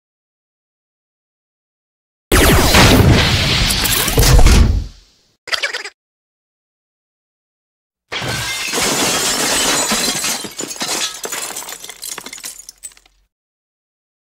Henry and Zak - S1:E6 - Evil Nothing.
Henry finds a battle ship and Zak destroys it.